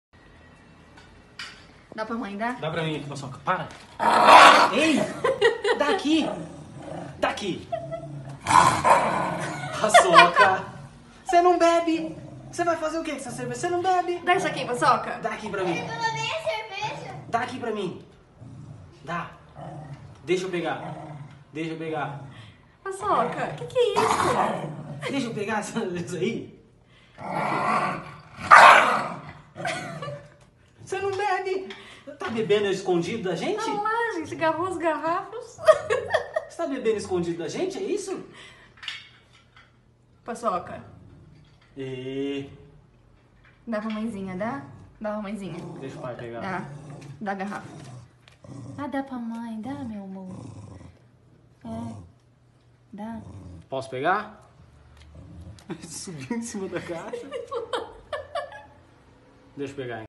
Cachorro defende caixa de cerveja heineken e fica bravo com o dono
Cachorro não deixa dono pegar caixa de cerveja heineken, defende com unhas e dentes.